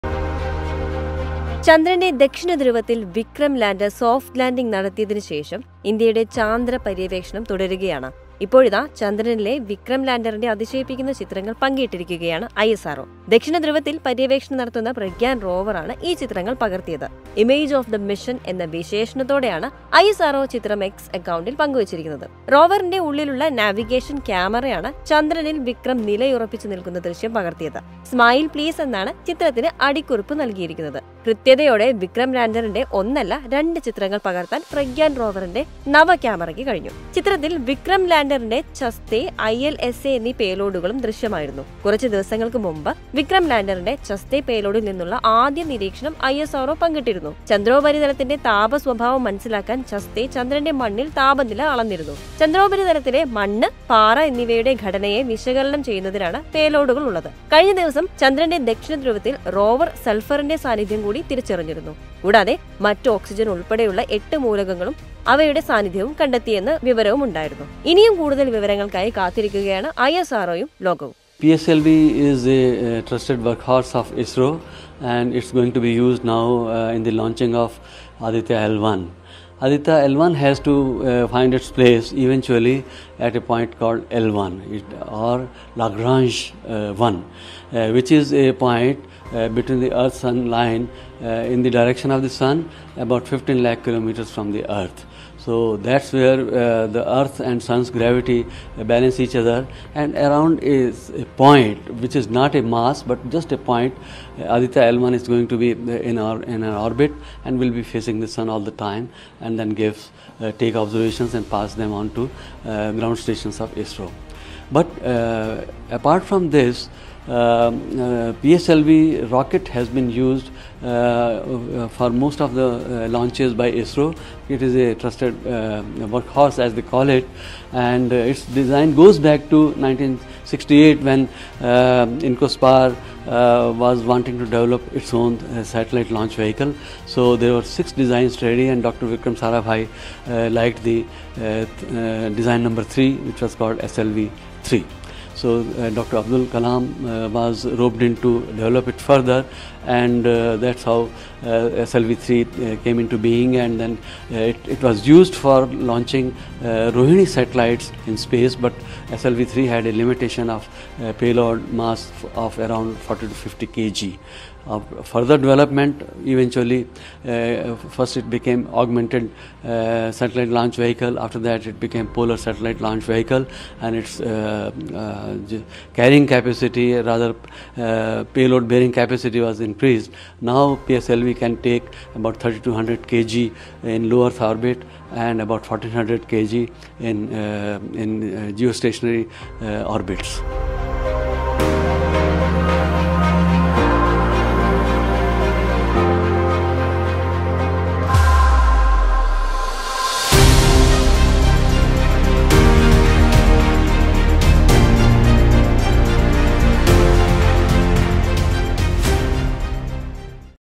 റോവർ പ്രഗ്യാൻ പകർത്തിയ വിക്രം ലാൻഡറിന്റെ ചിത്രങ്ങൾ പങ്കുവെച്ച് ISRO
“Smile Please!” Pragyan rover clicks Vikram Lander on Moon, ISRO shares stunning pictures

ചന്ദ്രനിലെ വിക്രം ലാൻഡറിന്റെ അതിശയിപ്പിക്കുന്ന ചിത്രങ്ങൾ  പങ്കിട്ടിരിക്കുകയാണ് ISRO.  ദക്ഷിണധ്രുവത്തിൽ പര്യവേക്ഷണം നടത്തുന്ന പ്രഗ്യാൻ റോവർ ആണ് ഈ ചിത്രങ്ങൾ പകർത്തിയത്.